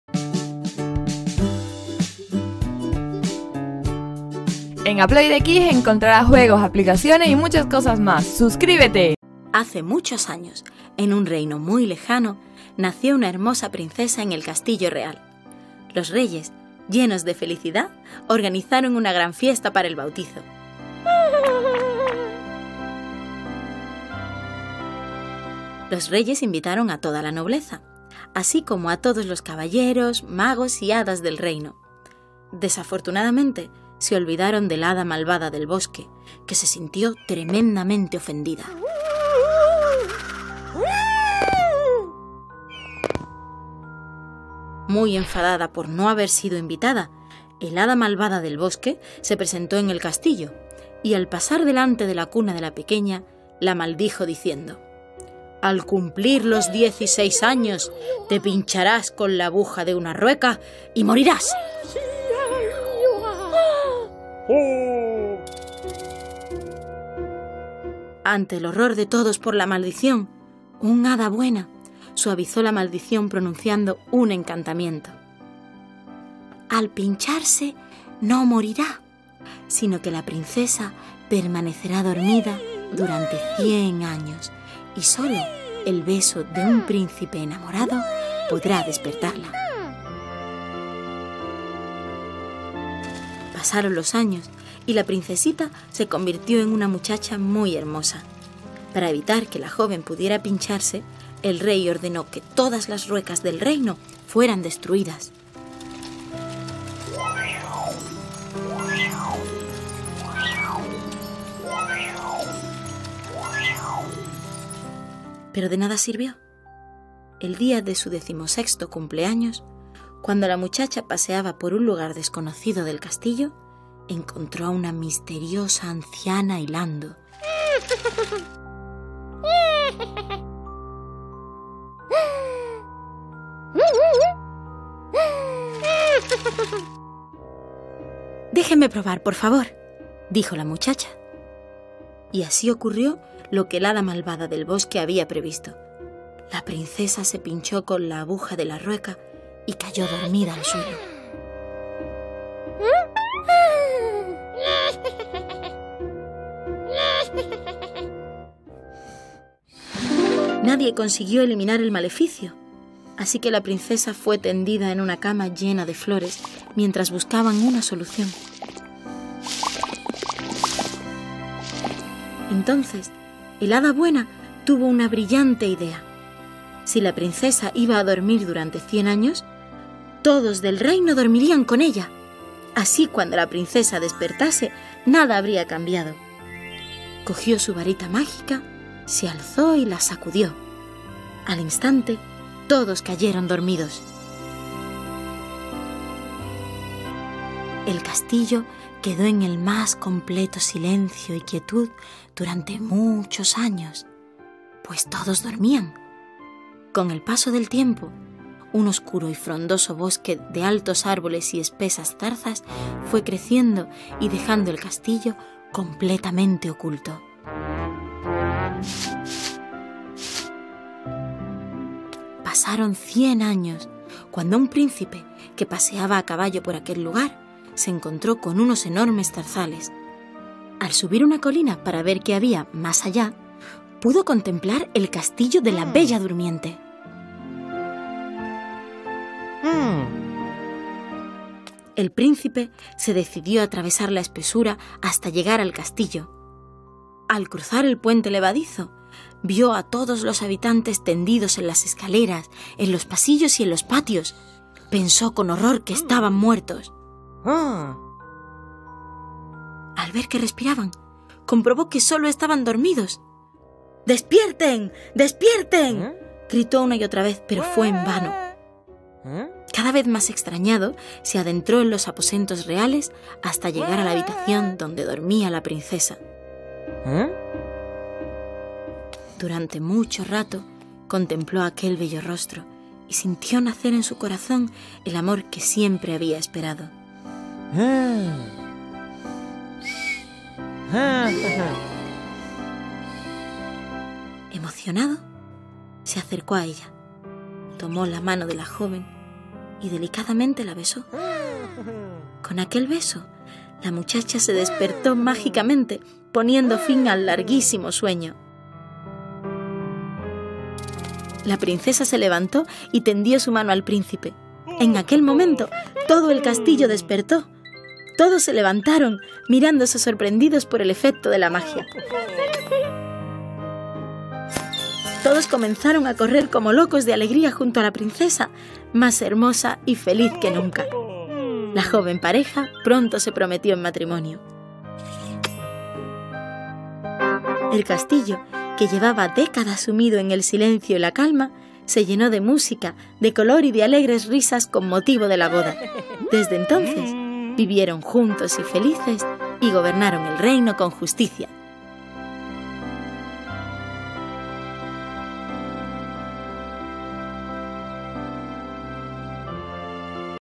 LA BELLA DURMIENTE - AUDIO CUENTO PARA NIÑOS | ESPAÑOL
Audio Cuento para niños LA BELLA DURMIENTE narrado, aunque puede leerse pausando en cada página. Estos cuentos pertenecen a la App para Android e iOS "Playkids Stories" que podéis descargar a continuación

INFORMACIÓN SOBRE LA APP
☉ App : Playkids Stories
☉ Desarrollador : Playkids Inc